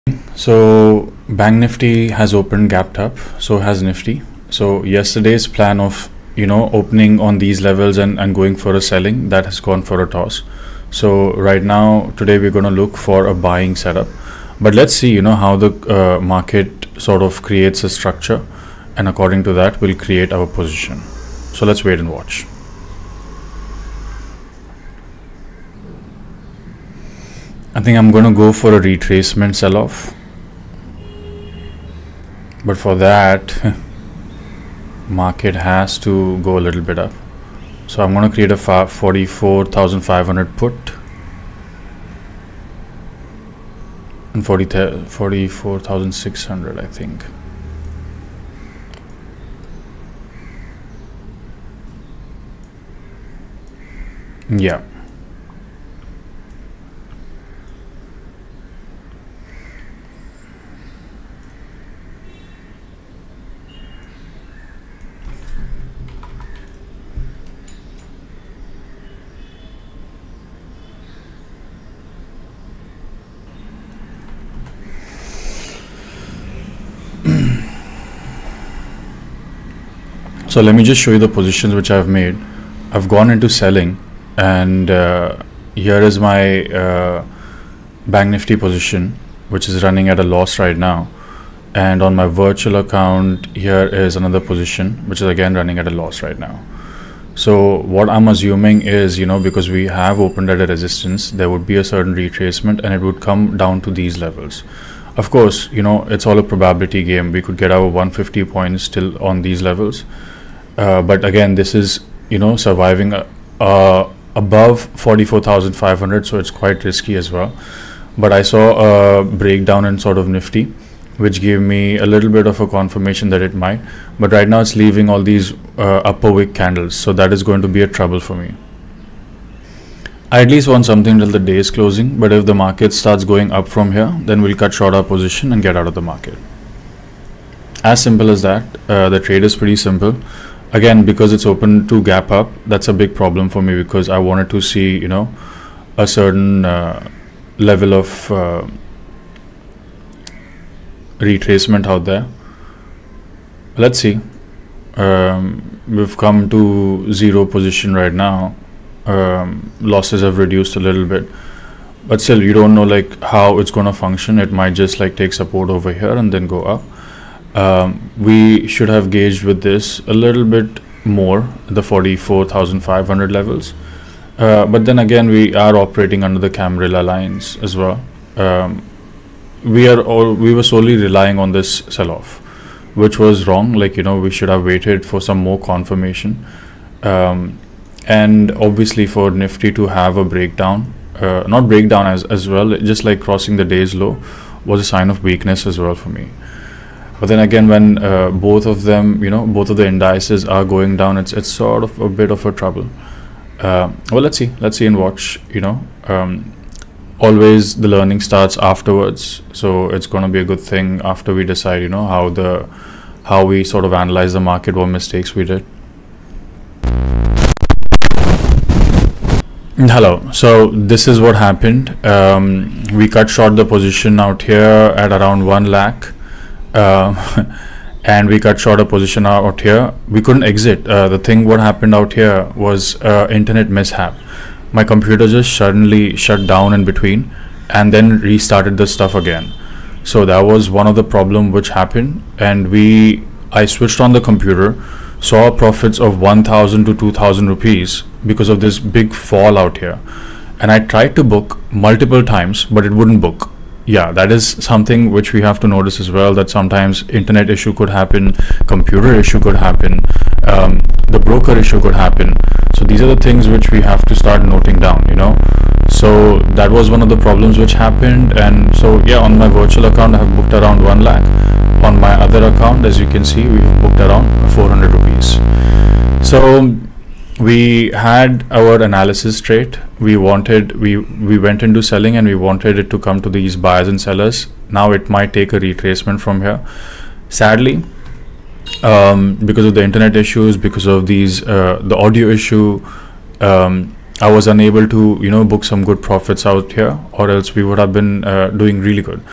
1 lot, 1 trade, 1 hour a day Live Intraday Trade banknifty options trading | September 4 2023
Tracking and documenting myself as I transition from an option seller to an option buyer.
And try to test a startegy that would just take an hour a day and give us enough time for life!